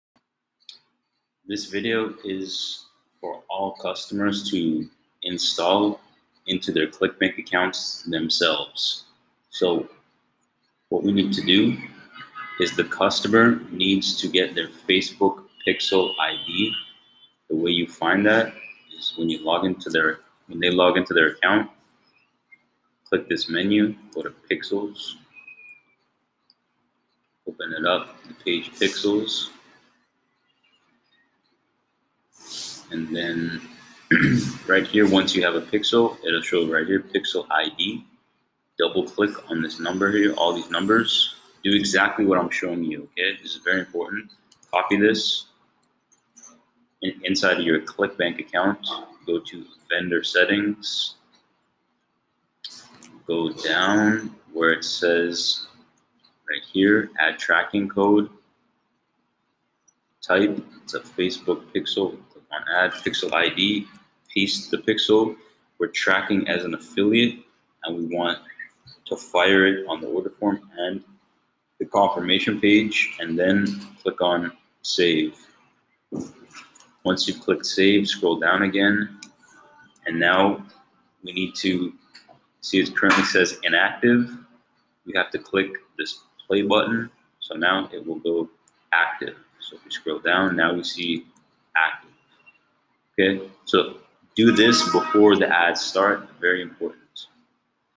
How to Add Facebook PIXEL to your ClickBank Account?
This video explains the procedure to add your facebook pixel ID into your clickbank account for conversion tracking.